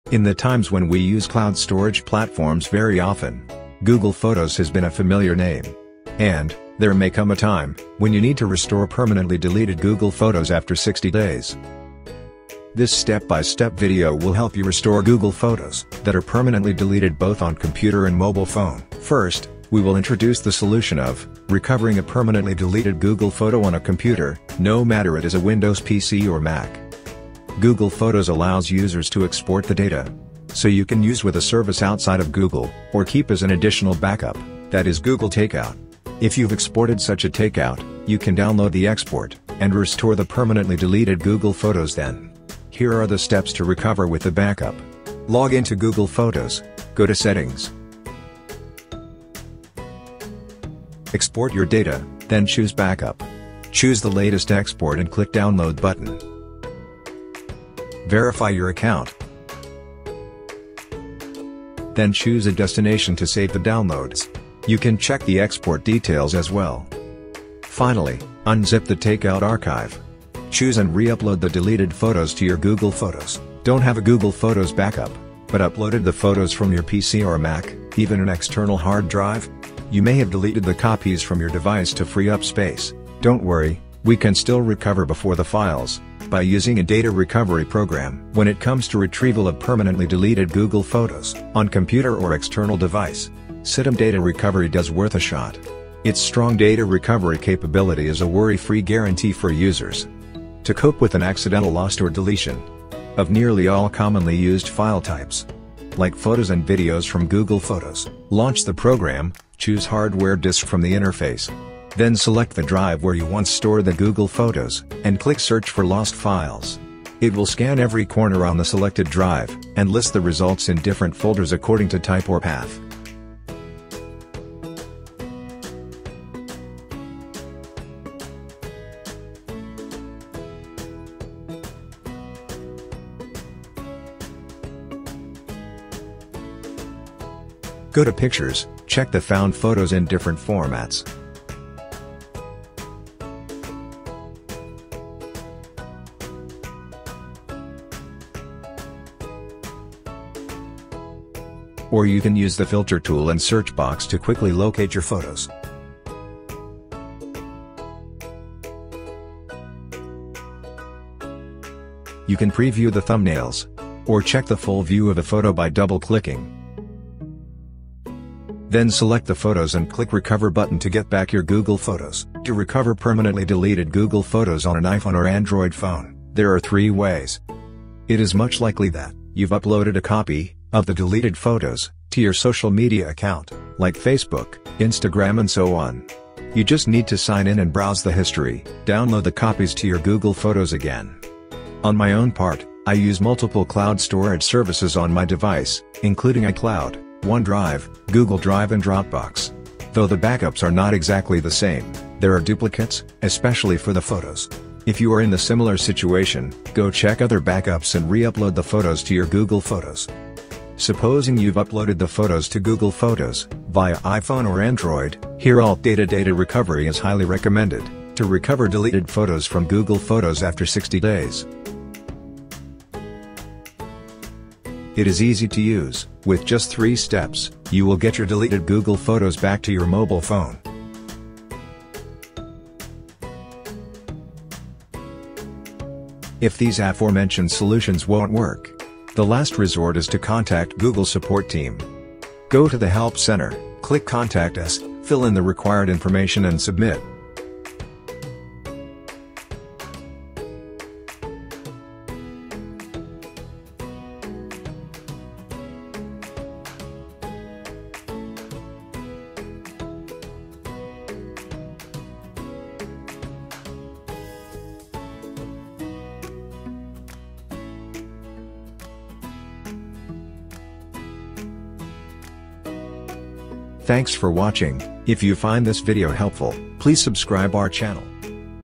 How to Recover Permanently Deleted Photos from Google Photos on Computer or Phone?
There may come a tim when you need to restore permanently deleted photos from Google photos after 60 days, this step-by-step video will help you restore Google photos that are permanently deleted on a computer or on a mobile phone, according to if you have a backup or not.
From 00:19, Part 1. How to recover permanently deleted photos from Google Photos on computer?
From 02:53, Part 2. How to recover deleted Google Photos after 60 days on Android or iPhone?
Also, you can check the full guide here to recover Google photos